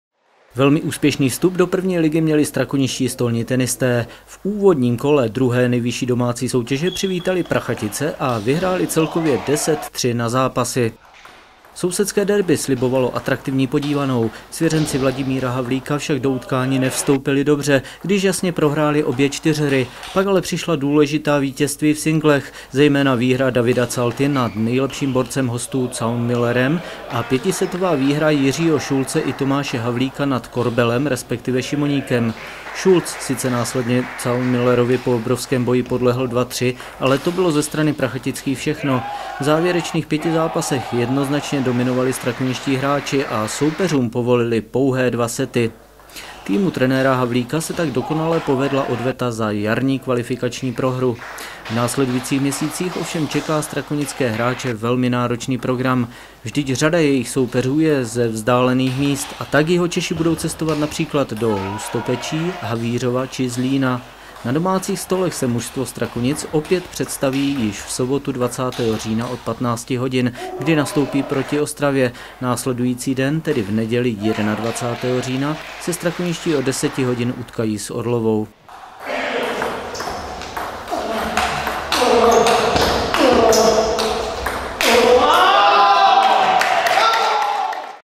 Stolní tenisté deklasovali Prachatice
Velmi úspěšný vstup do 1. ligy měli strakoničtí stolní tenisté. V úvodním kole druhé nejvyšší domácí soutěže přivítali Prachatice a vyhráli celkově 10 : 3 na zápasy.